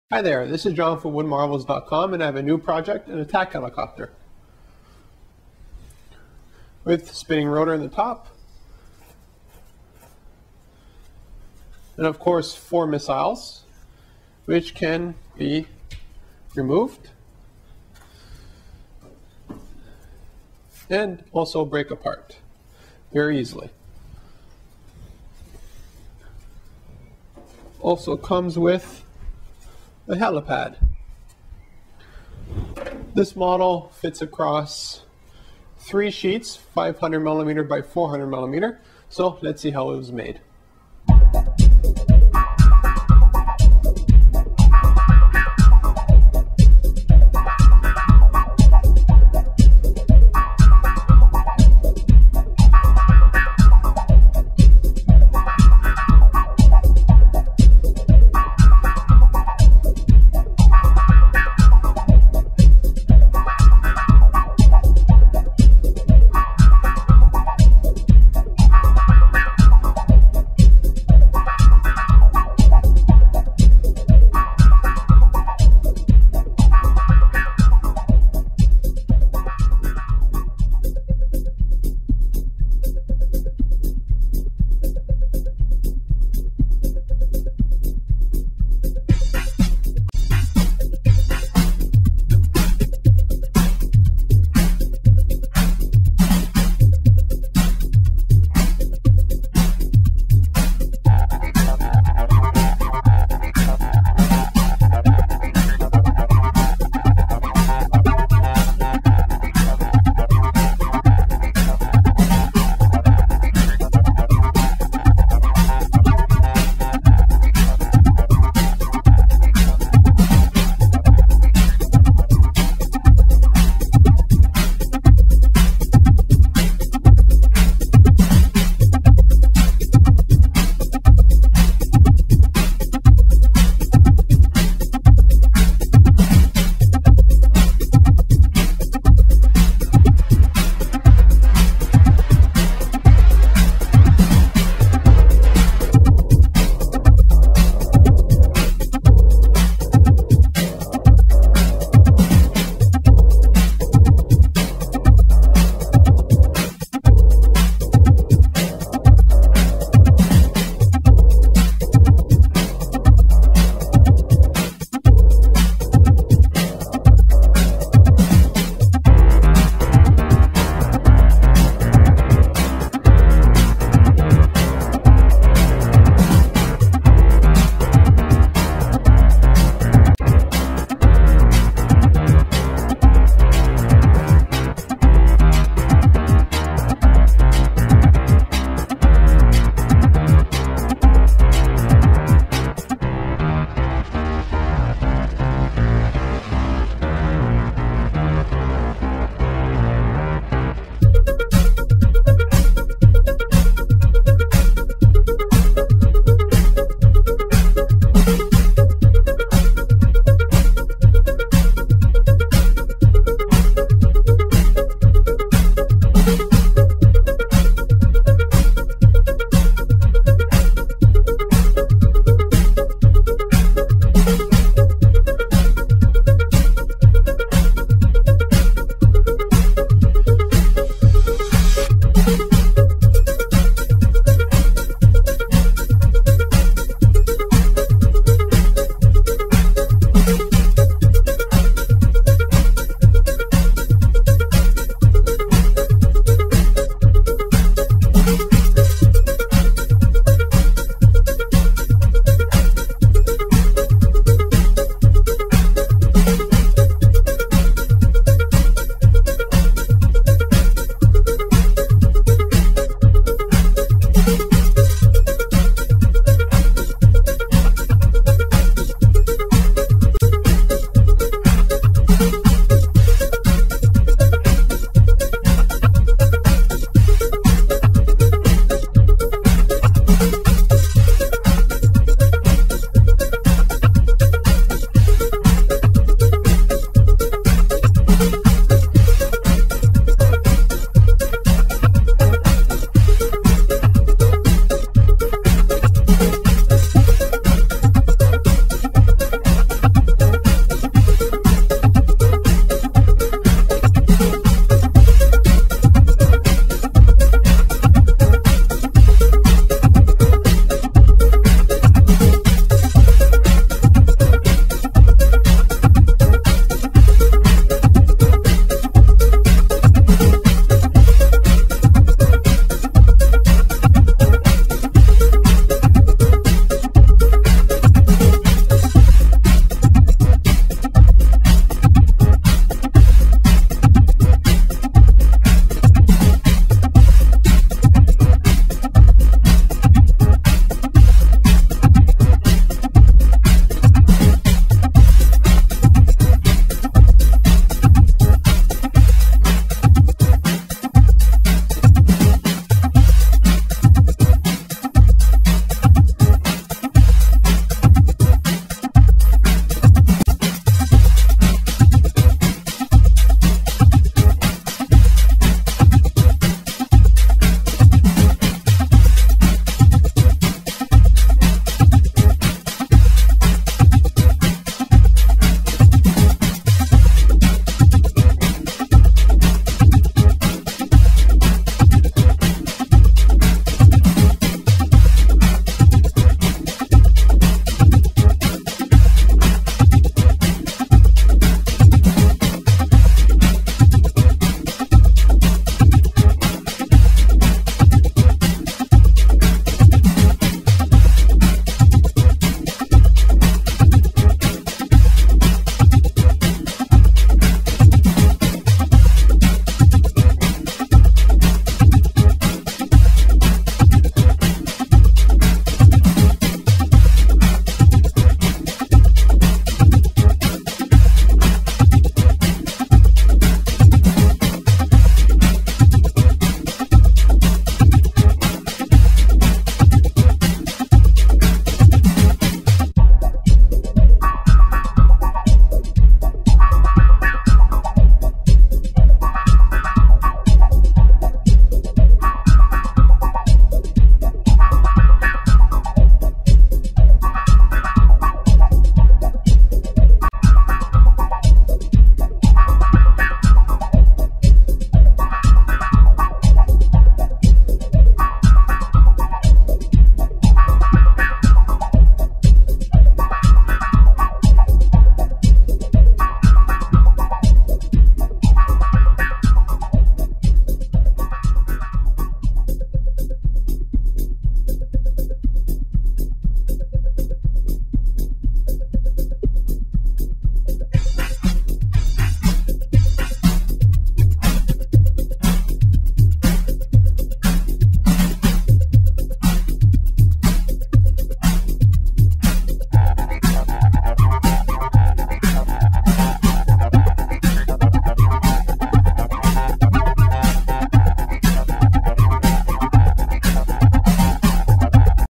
ShopBot'ing an Attack Helicopter: Proof of Concept
This video shows me cutting (with a Desktop ShopBot) and assembling a CNC Attack Helicopter. You can buy this digital CNC plan and many others at WoodMarvels.com.